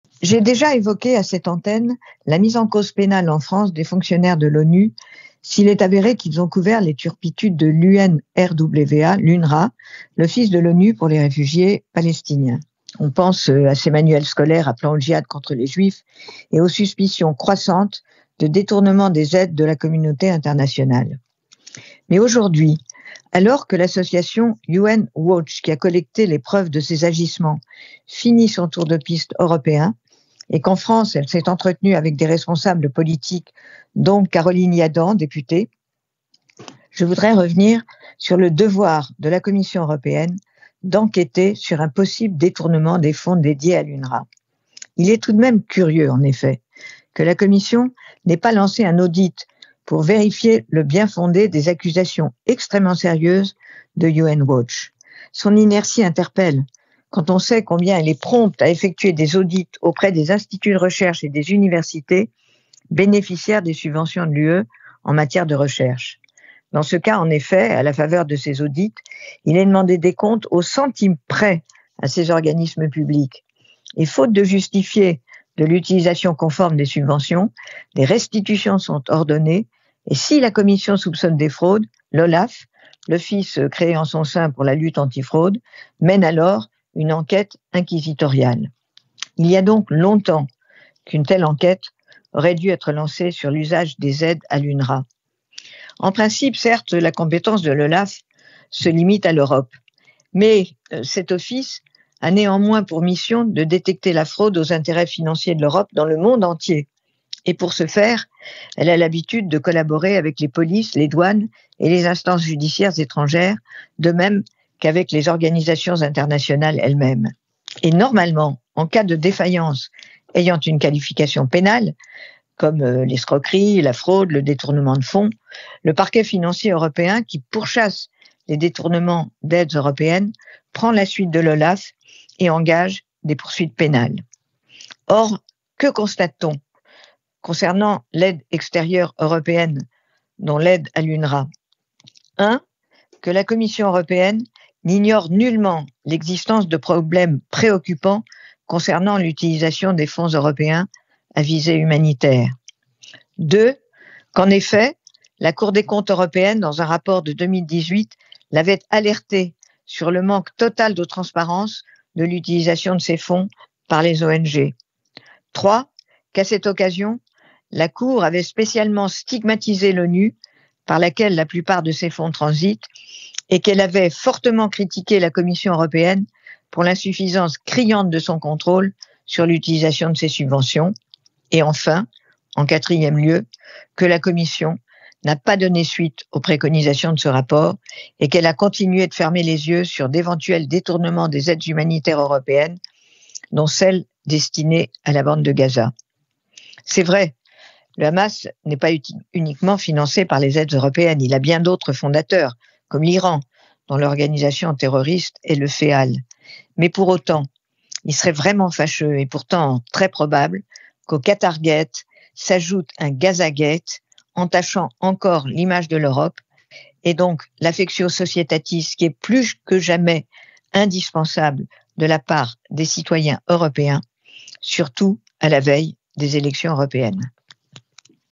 L’Europe face aux accusations sérieuses d’UN Watch sur les aides à l’UNRWA
Le billet d’humeur de Noëlle Lenoir